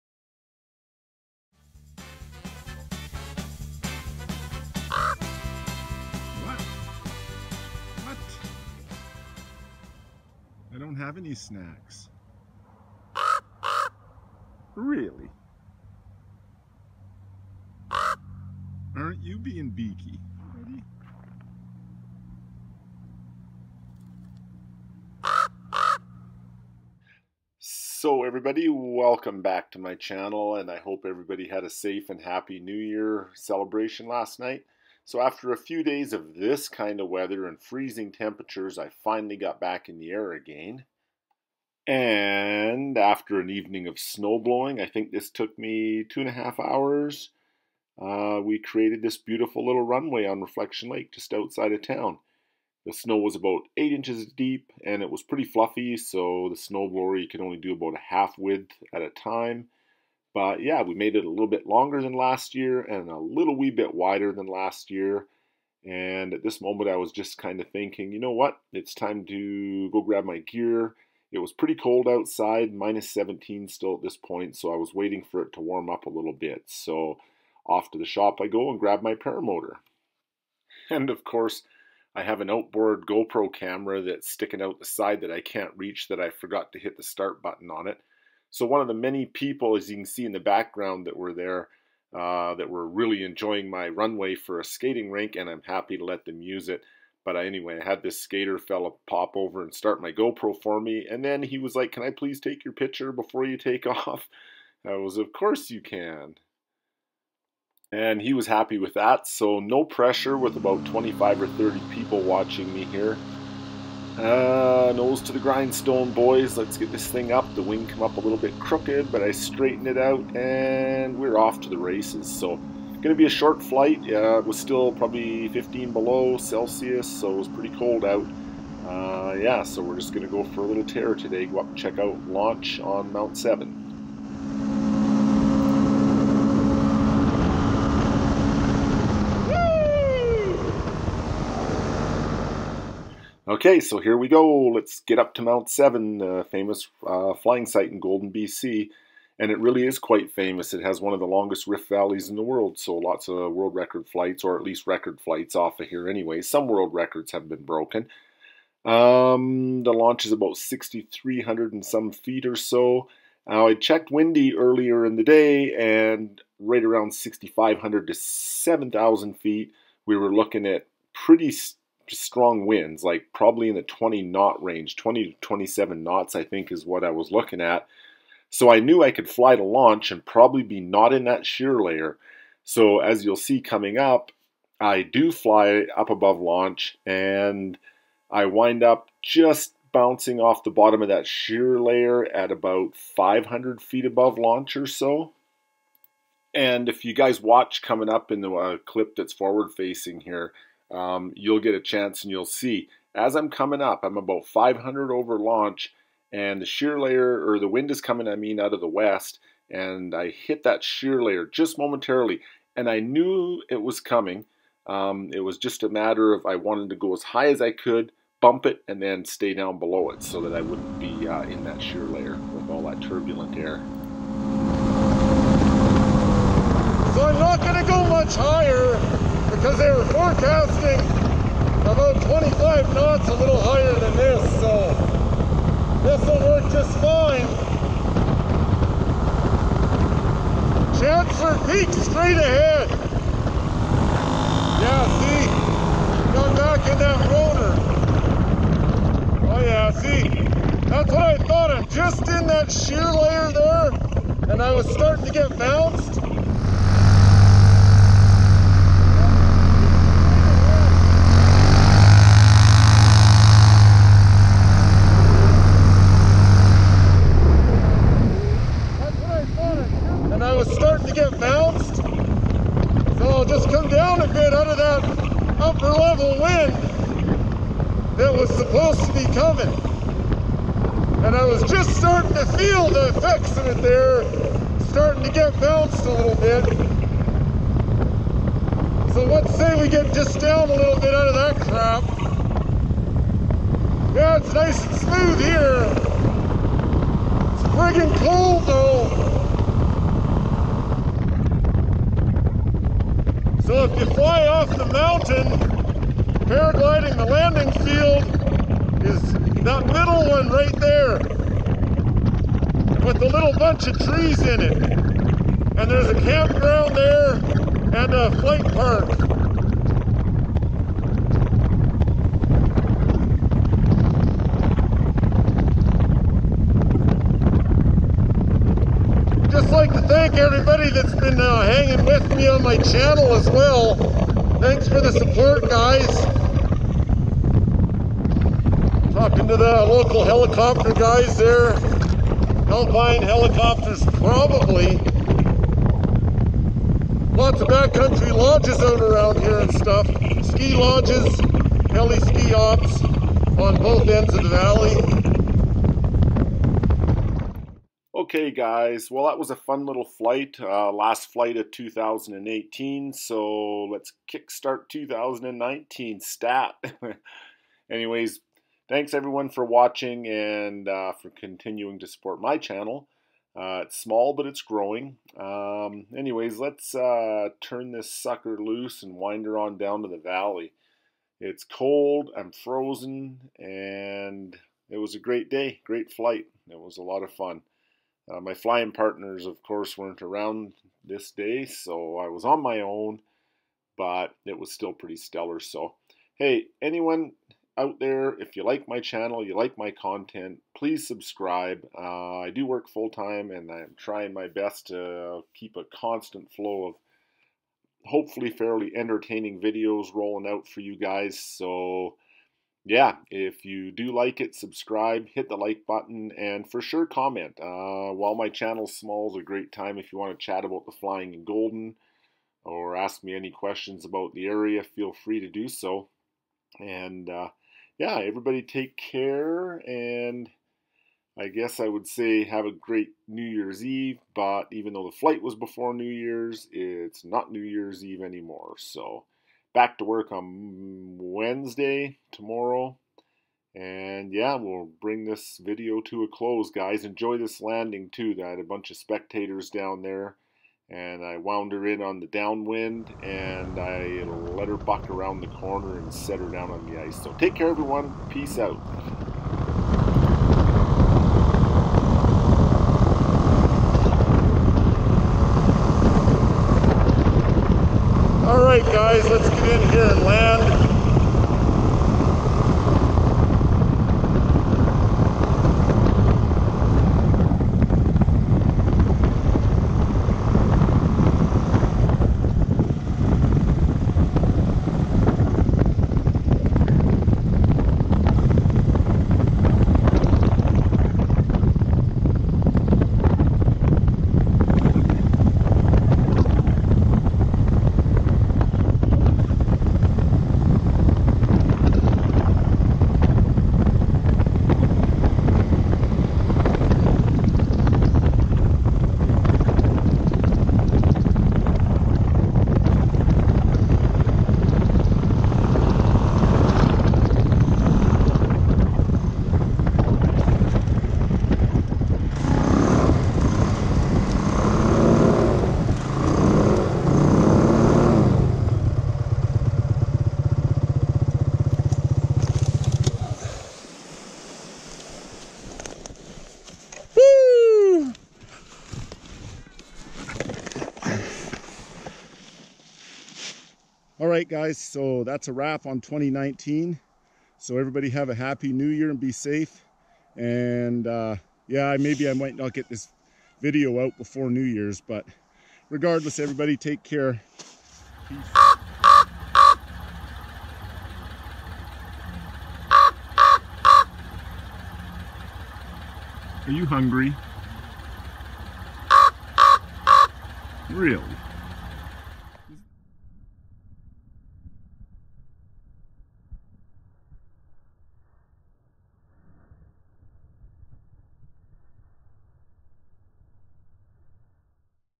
Big storm! Then cracks blue for a stunning flight!!!
Beautiful B.C. Getting the last of the goods for 2018